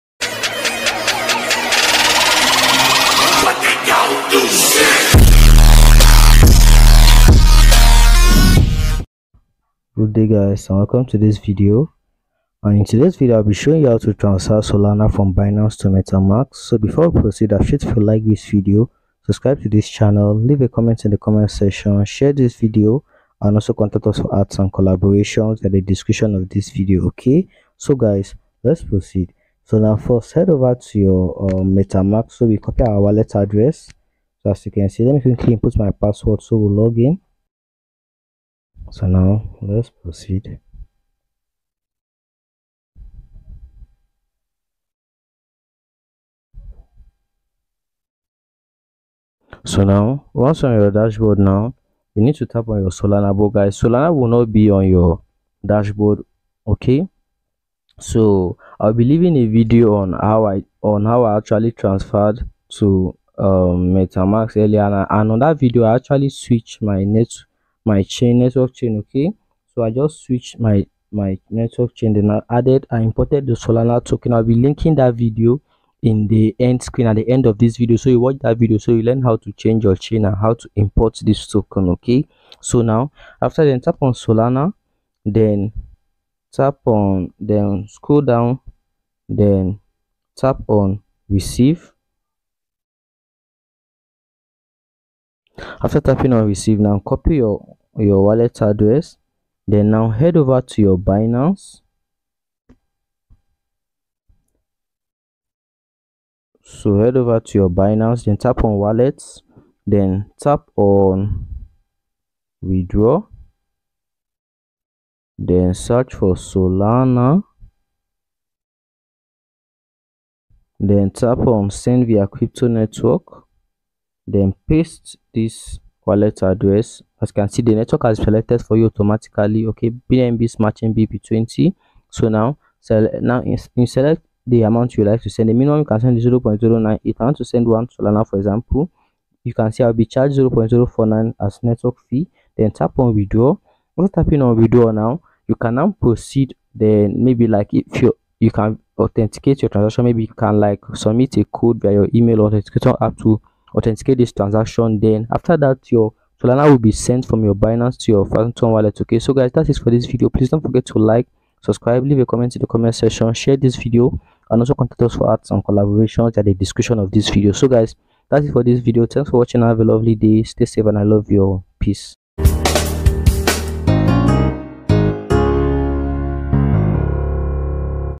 How To Transfer Solana From Binance To Metamask
In this step-by-step tutorial, learn how to transfer Solana from Binance to Metamask seamlessly. Whether you are new to cryptocurrency or a seasoned investor, this complete guide and tips will walk you through the process of moving your Solana tokens securely.

🎯 Watch now to ensure a smooth transition from Binance to Metamask and manage your assets with ease. Don't miss out on this essential guide for transferring Solana from Binance to Metamask!

?sub_confirmation=1

Twitter Handle

🔑 Related Keywords 🔑